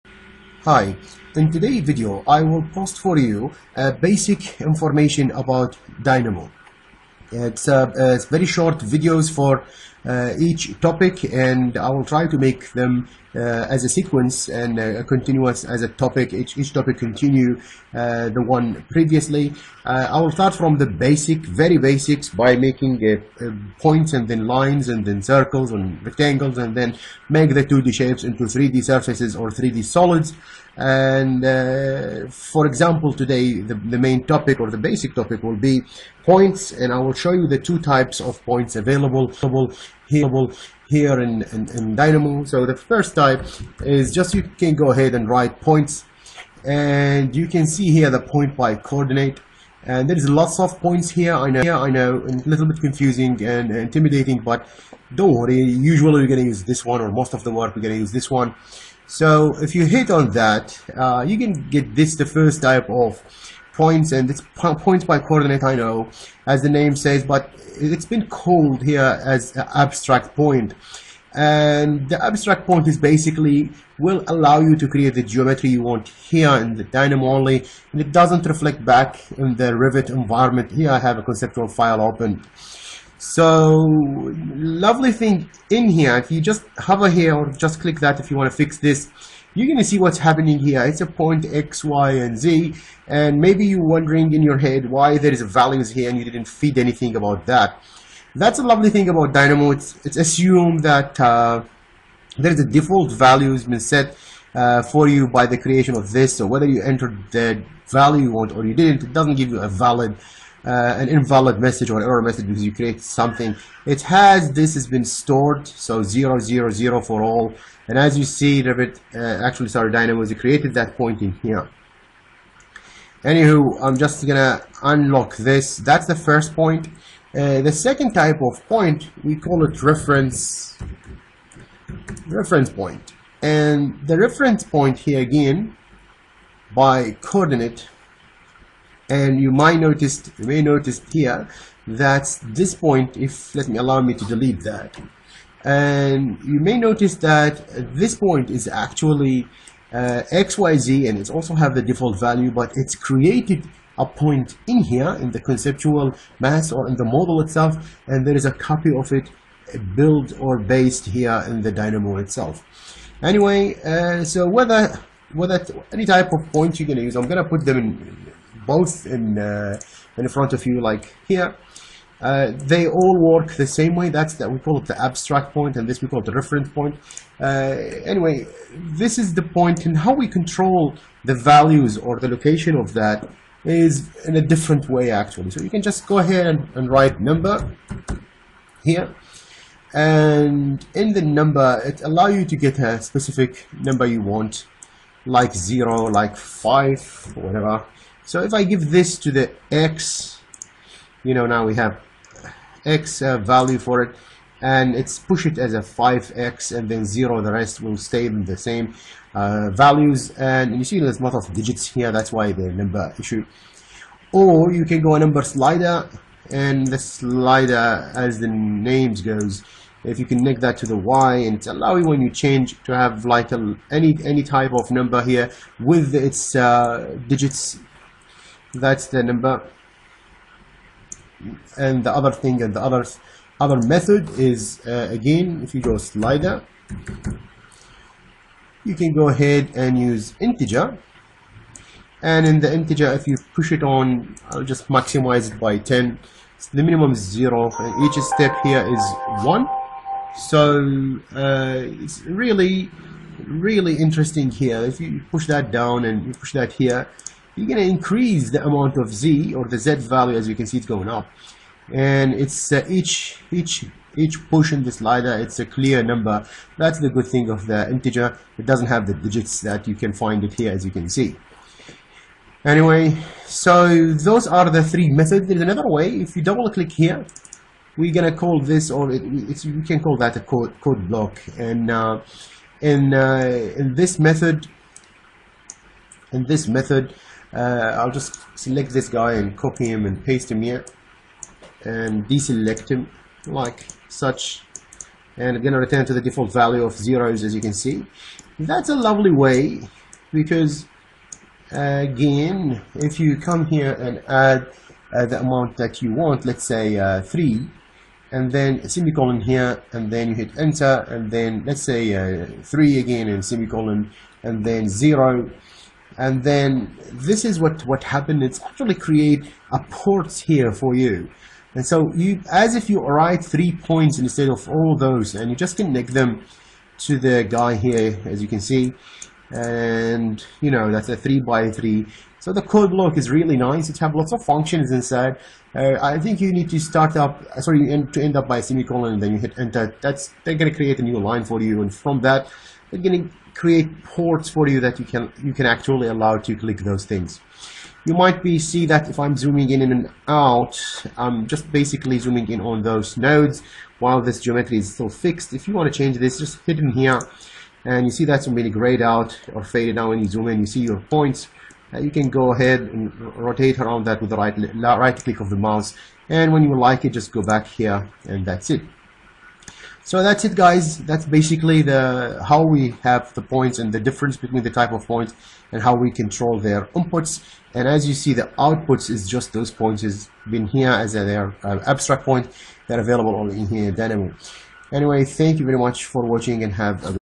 Points Types in Dynamo _ Reference V.S Abstract Points
Making Points in Dynamo

1- Abstract Points

2- Reference Points

3- Input methods